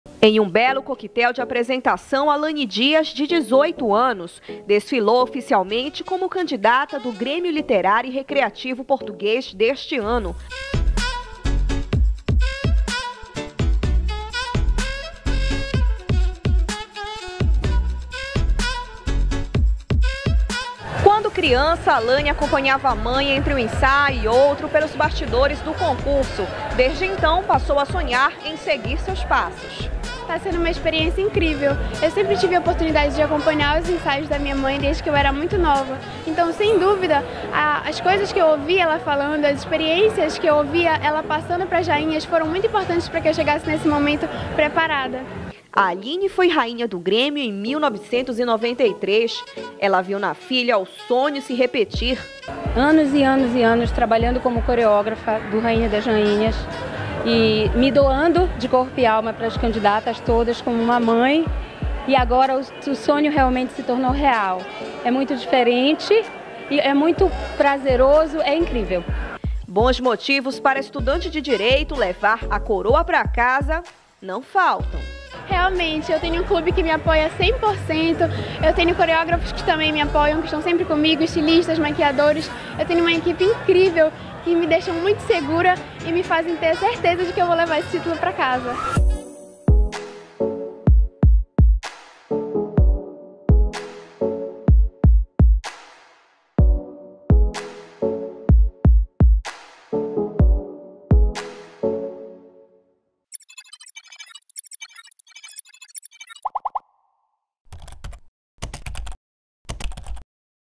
Entrevista de Alane Dias, do BBB 24, no Rainha das Rainhas
Mãe de Alane Dias, que também venceu o concurso mais tradicional do Pará, conta o que fez para ajudar a filha a vencer o Rainha das Rainhas 2018